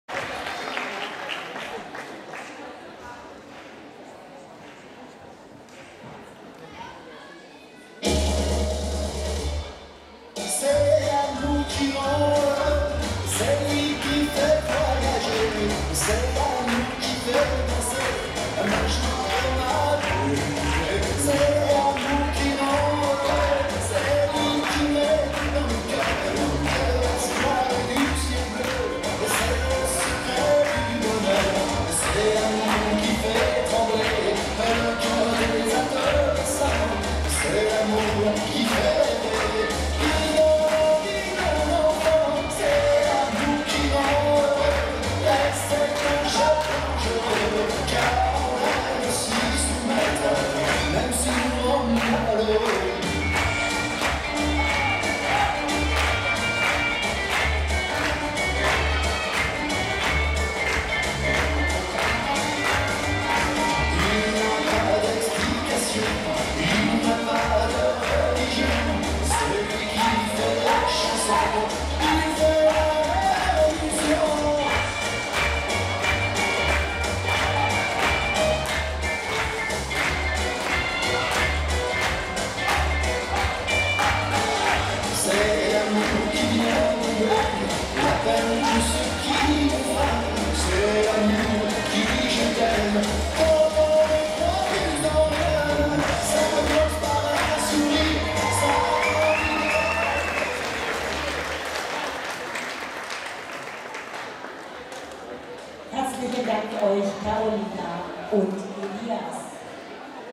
RR Beginners, Final/4, SRRC Schweizermeisterschaft 2019, Schaffhausen
"Rang 2 - Vize-Schweizermeister
492 Elias ORTEGA - Karolina HEIT (Rock Academy)
Rock 'n' Roll-RR Beginners, Final, 4. Heat
SRRC Rock 'n' Roll Schweizermeisterschaft 2019, Championat Suisse
Rock 'n' Roll Club Angeli Neuhausen in Schaffhausen am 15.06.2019"